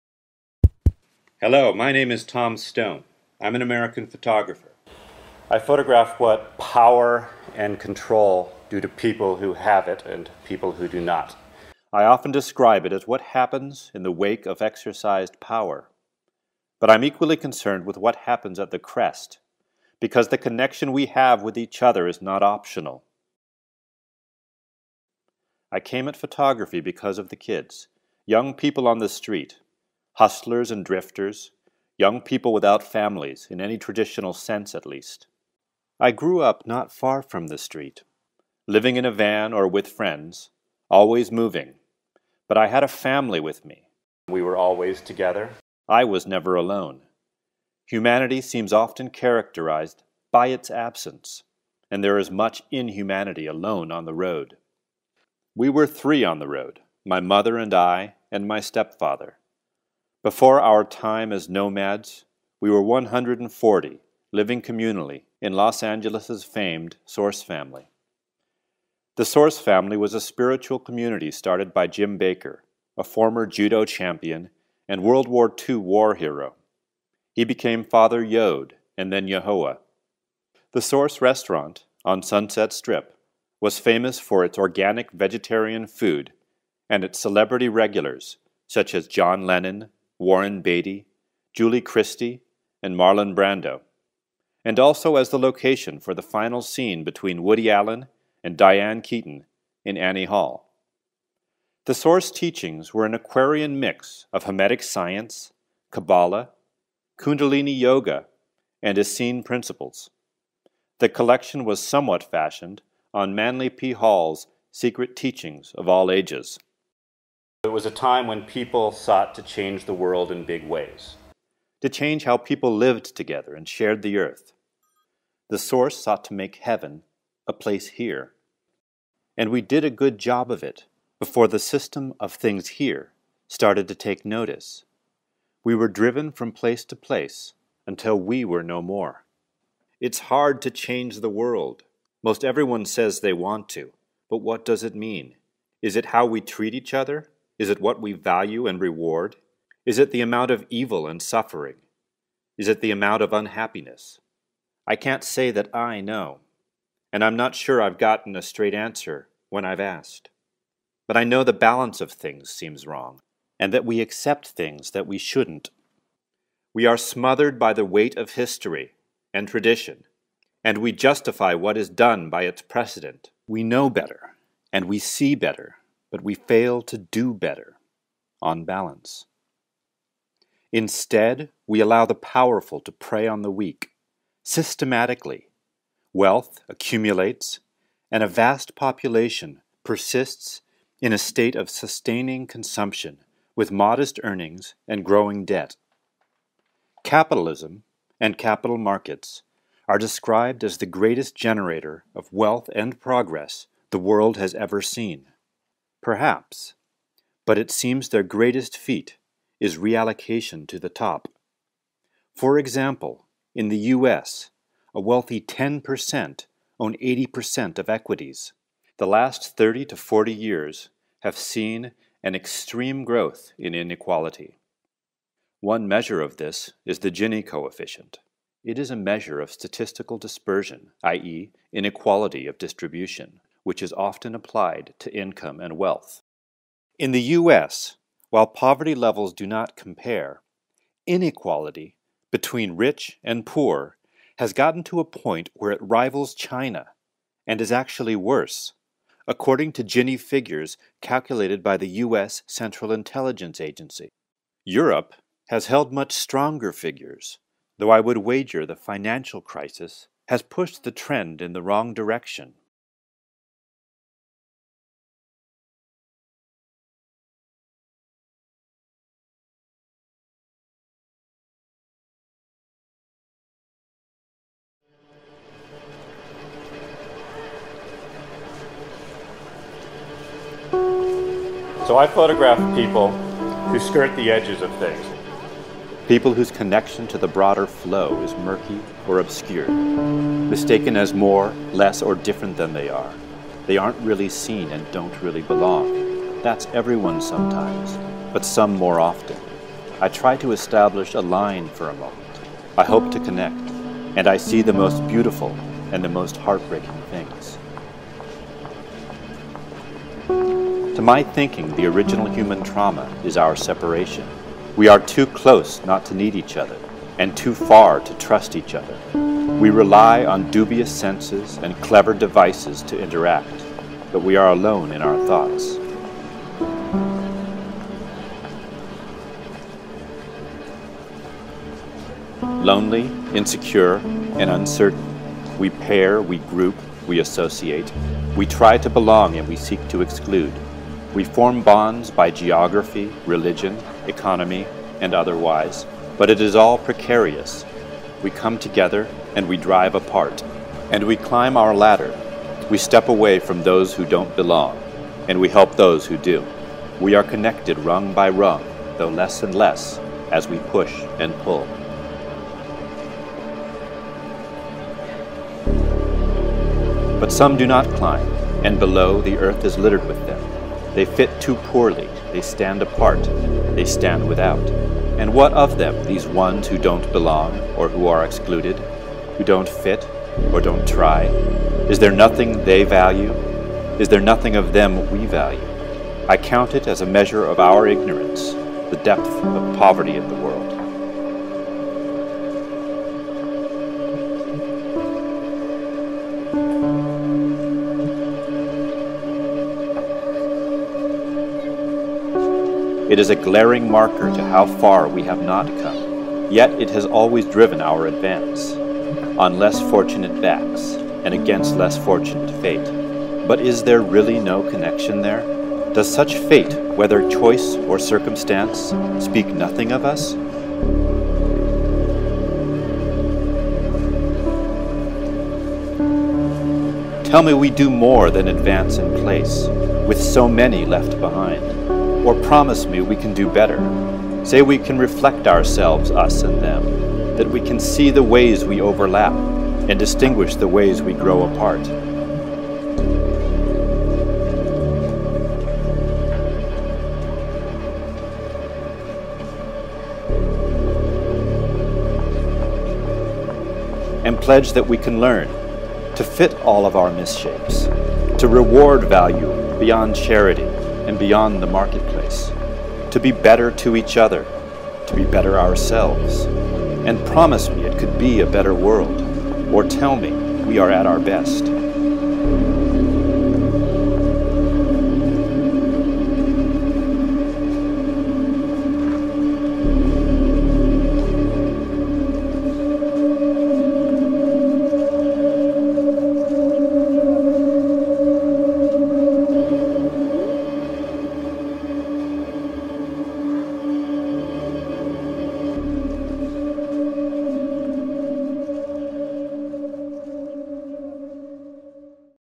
Tom Stone: Artist Statement, University of Freiburg, 2011
Photographer Tom Stone reads his Artist Statement at the conference "Arm, nicht sexy" at the University of Freiburg in July 2011